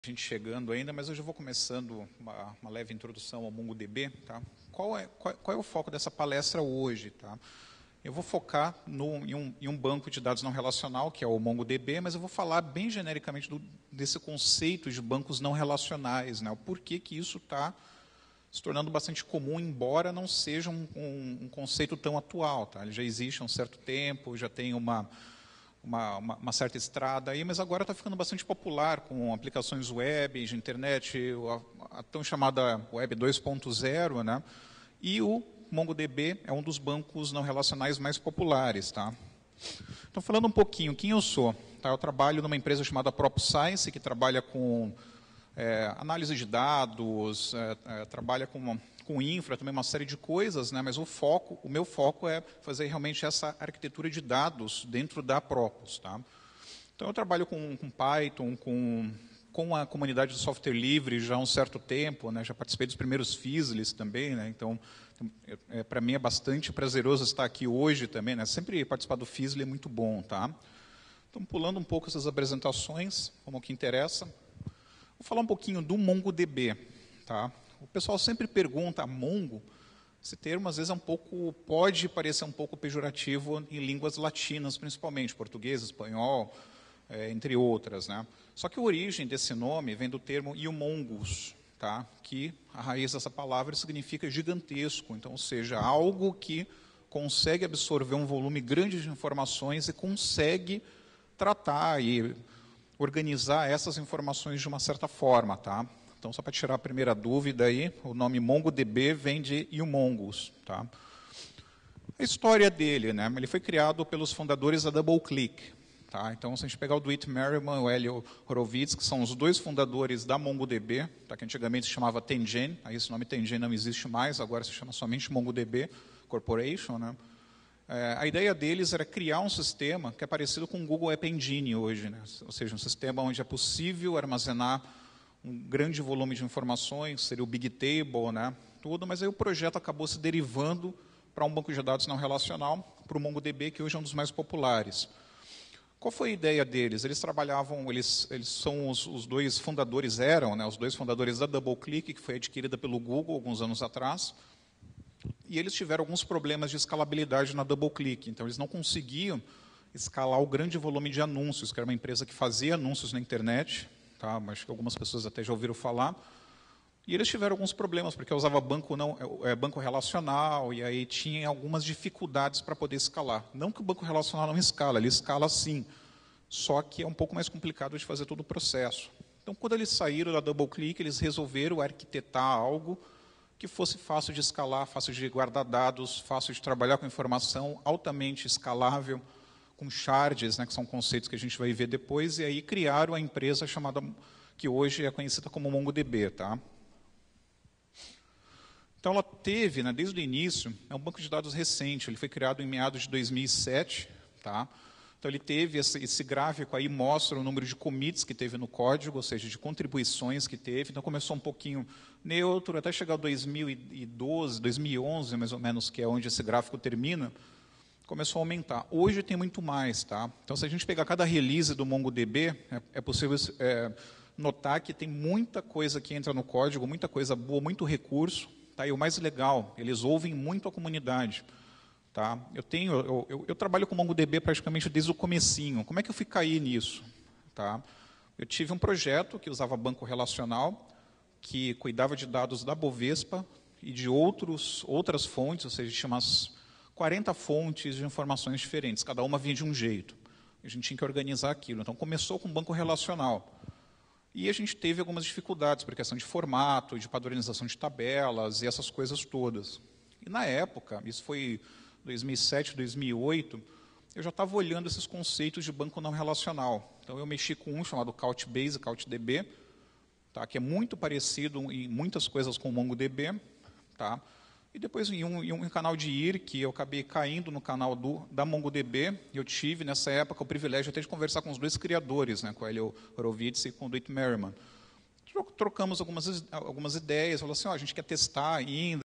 MongoDB e NoSQL, tudo o que você precisa saber
Conceito de persistência no relacional ou NoSQL se tornou algo bastante popular hoje em dia. MongoDB  um dos NoSQL mais populares, possui uma curva de aprendizado relativamente pequena. Nessa palestra, todos os principais conceitos serão apresentados, assim como as principais novidades do MongoDB.  uma palestra indispensável para quem busca o caminho das pedras e  uma rápida introdução ao MongoDB

Christiano Anderson